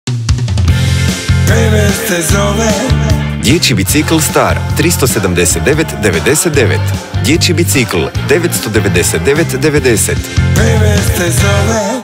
Ponuda vrijedi od 09.03.2017. do 29.03.2017. - 6
Dječji bicikl STAR 88x36,5x54 cm, za djecu 3-6 g. 379,99 kn

Dječji bicikl DINAMIC BOLT DIABLO veličina kotača: 20”, 6 brzina, V-brake kočnice, blatobrani. 999,90 kn

Ponuda vrijedi od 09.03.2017. do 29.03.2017. ili do isteka zaliha.